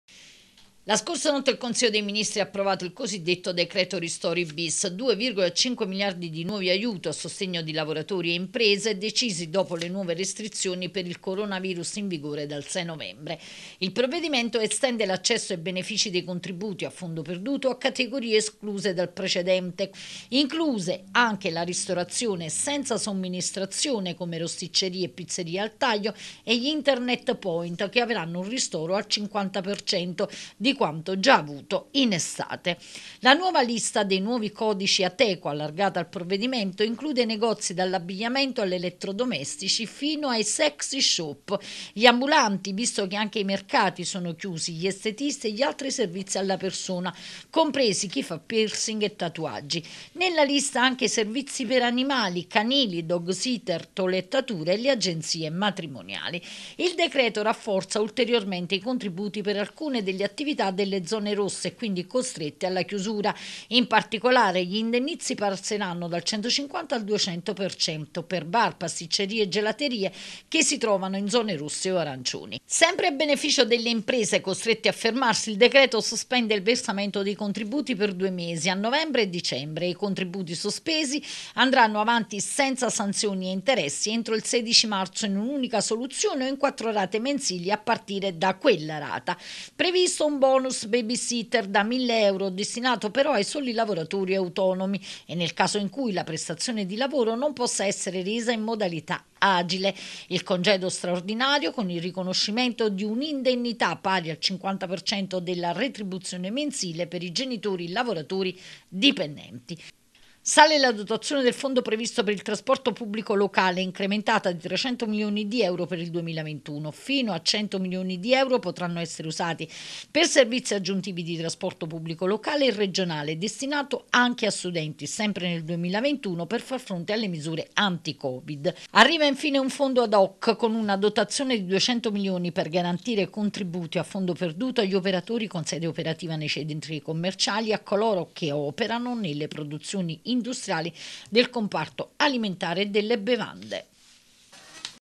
Il decreto ristori bis. Da tasse rinviate ai bonus sostegni per 2,5 miliardi
Il decreto approvato nella notte amplia la platea delle attività che potranno accedere agli indennizzi a fondo perduto e rafforza quelli per le attività chiuse nelle zone rosse. Maxi rinvio delle imposte per le imprese colpite.